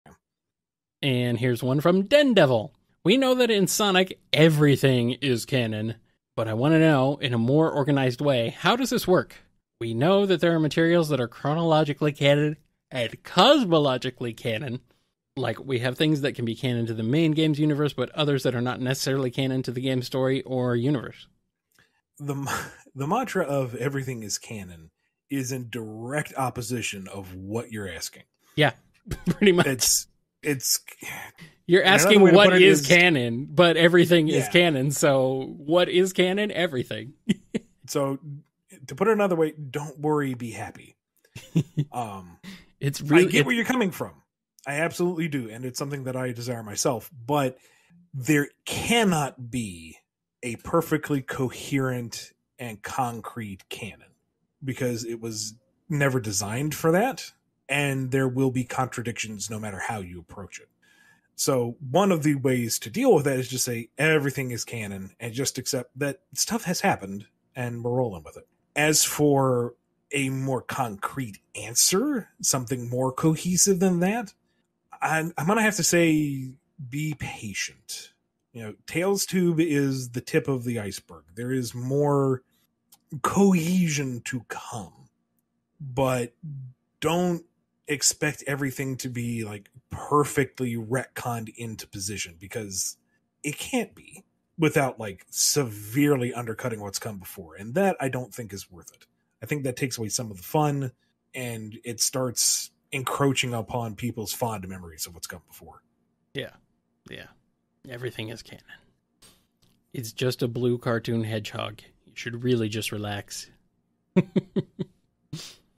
Ian Flynn confirms Everything Being Canon in Sonic
I told people that SEGA was in on this shit and that it was over Ian.

If everything is canon including the entire cosmology then Ian saying the other sonic media are parallel universes begins making sense.